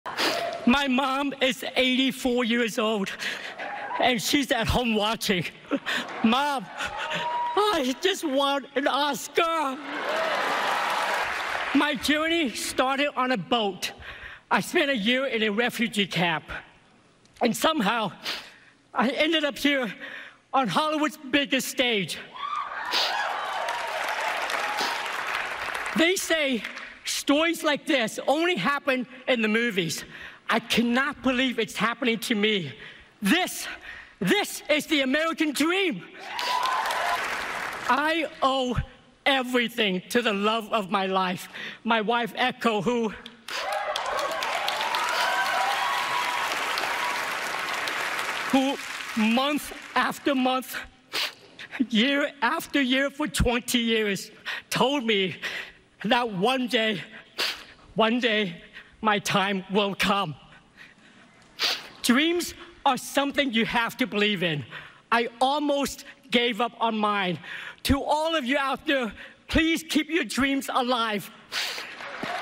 El emocionado discurso de Ke Huy Quan en los Oscar
El ganador del premio Oscar al Mejor Actor de Reparto 2023, Ke Huy Quan, dio uno de los discursos más emocionantes de la noche. Esto fue lo que dijo